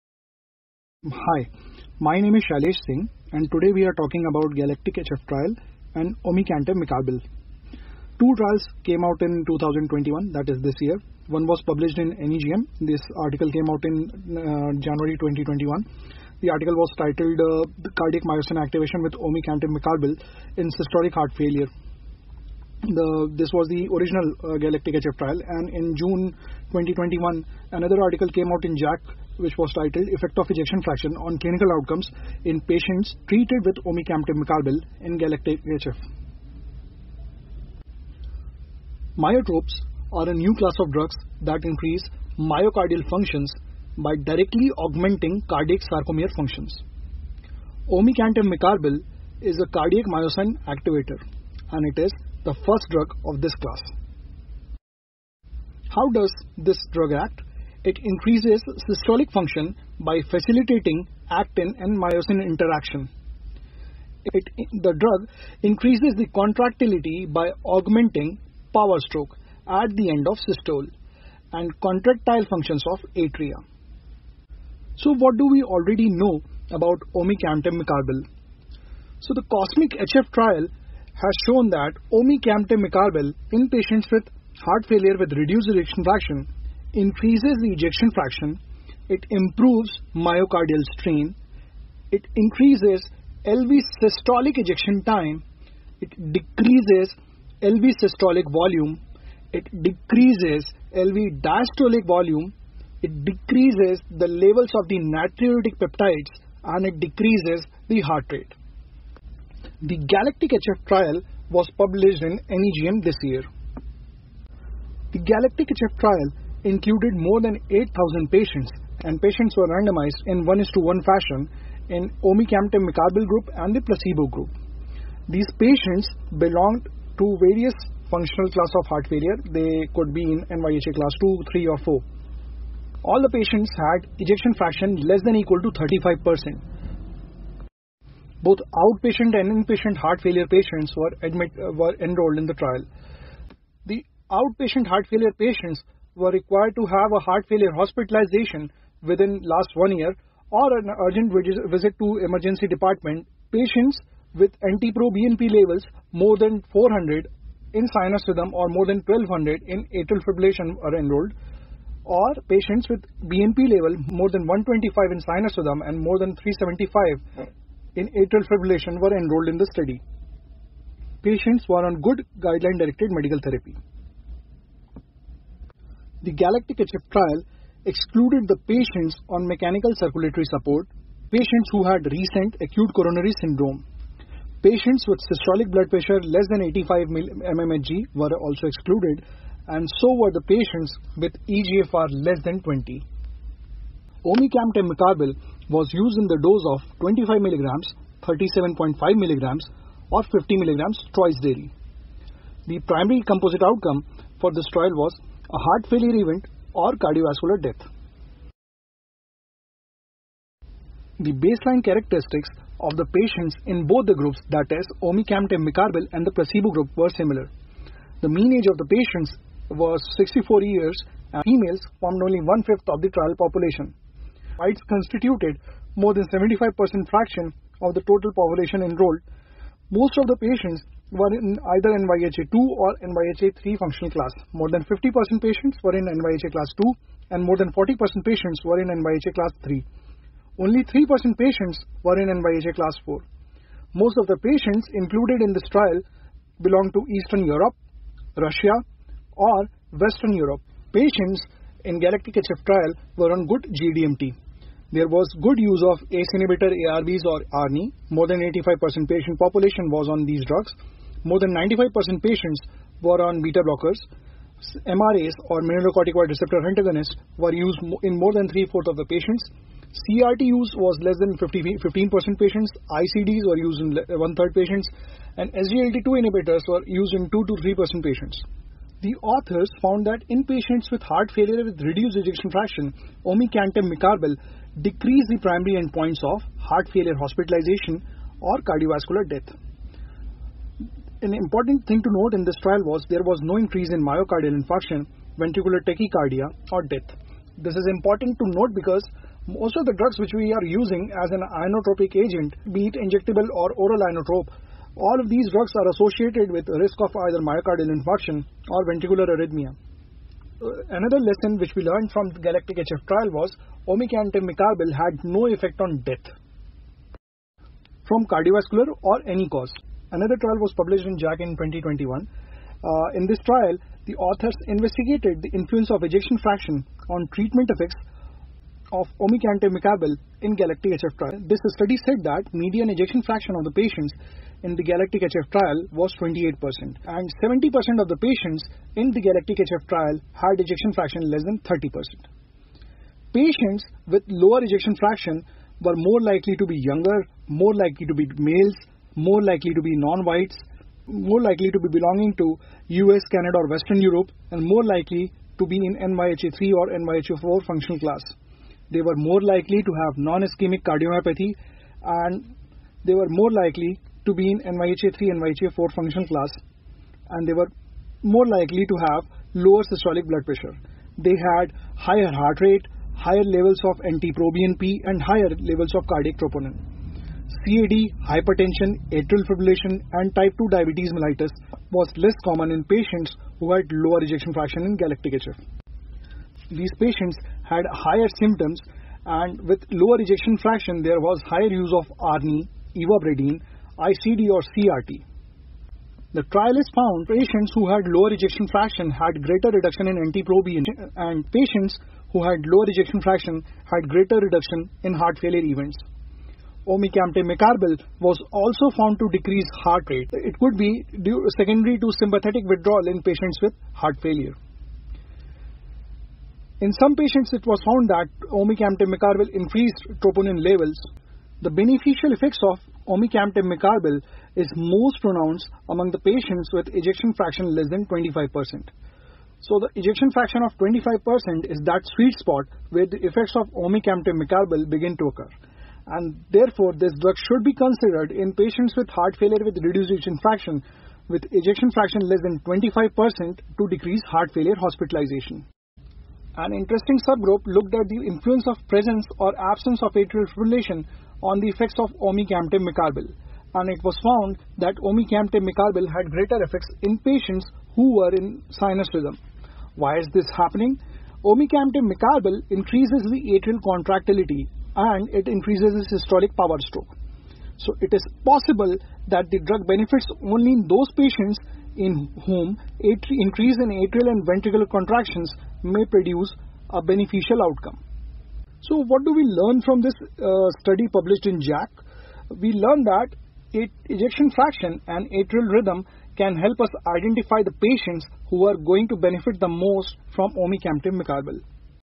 GALACTIC - HF & OMECAMTIV MECARBIL. JACC 2021. NEJM 2021
Role of omecamtiv mecarbil in heart failure with reduced ejection fraction (HFrEF) was published in GALACTIC- HF trial (NEJM 2021). Recently, in JACC the effect of ejection fraction on response to Omecamtiv mecarbil in patients enrolled in GALACTIC-HF trial was published.
In this presentation I have discussed both these studies together.
Omecamtiv mecarbil may find its place in newer guidelines for HFrEF.
To know more about Omecamtiv mecarbil in patients with HFrEF and both the trials published in NEJM and JACC watch this video.